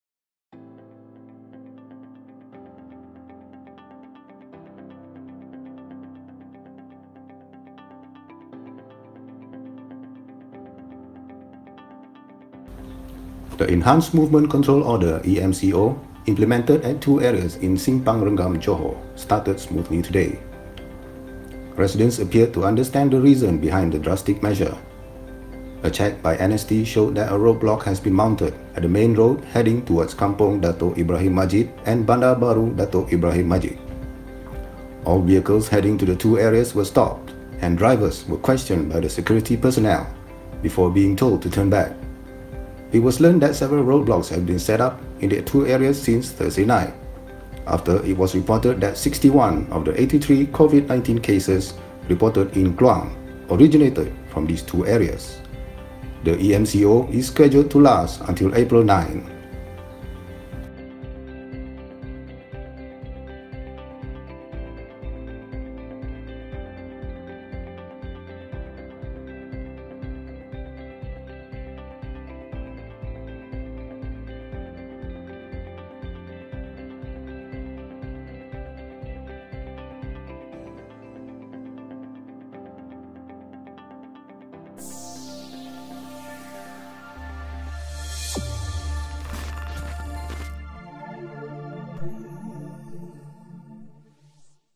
Smooth implementation of enhanced MCO in Simpang Renggam
The implementation of the Enhanced Movement Control Order (EMCO) in two areas in Simpang Renggam here started smoothly as residents and the general public seemed to understand the imposition of the order.

  A check by NST showed that a roadblock has been erected at the main road heading towards Kampung Datuk Ibrahim Majid and Bandar Baharu Datuk Ibrahim Majid.

  All vehicles heading towards the two areas were stopped and asked by the police and armed forces personnel manning the roadblock to turn back.

  All the drivers of the vehicles seemed to understand and turned back without too much protests.